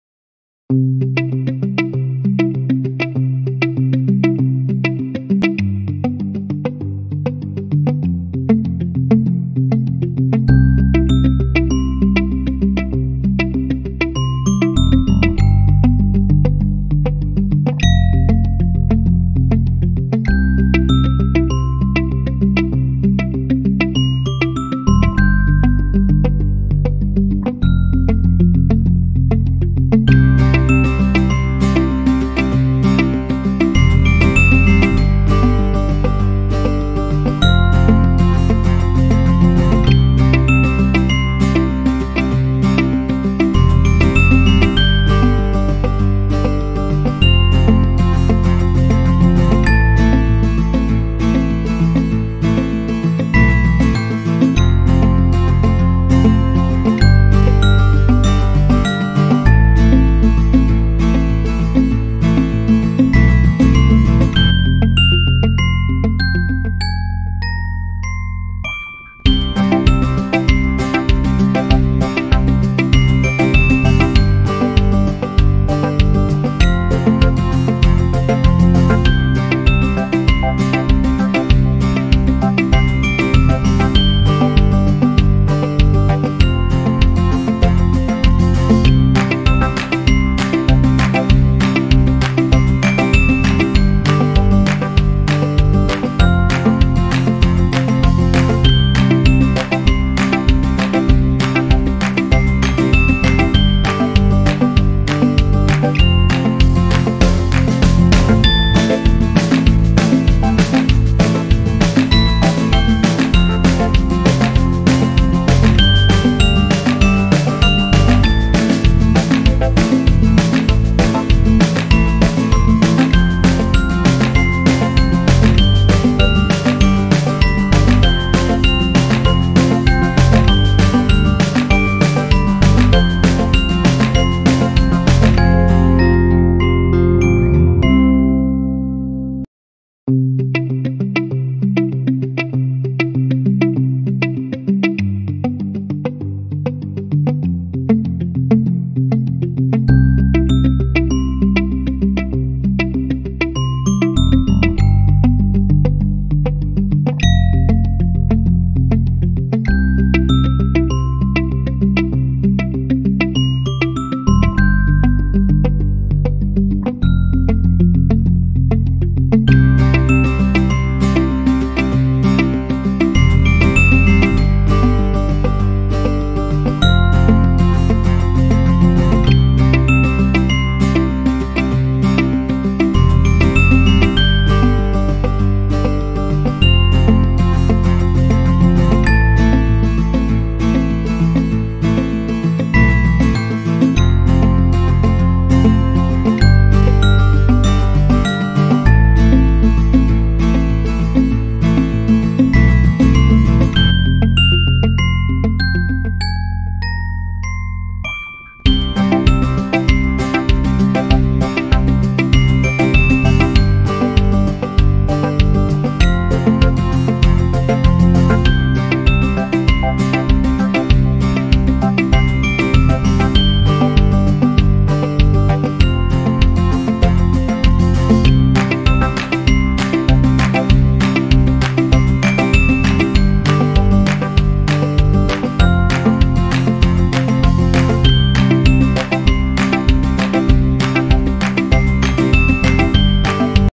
Guess the Traditional Games of TamilNadu | @riddleland2902
Try to guess the traditional game by connecting the images shown in the video 🖼➕🖼 You will find a game that may be your favourite game in your childhood days 💖🥰💜

Enjoy Finding 🥰💖❤

Level : Easy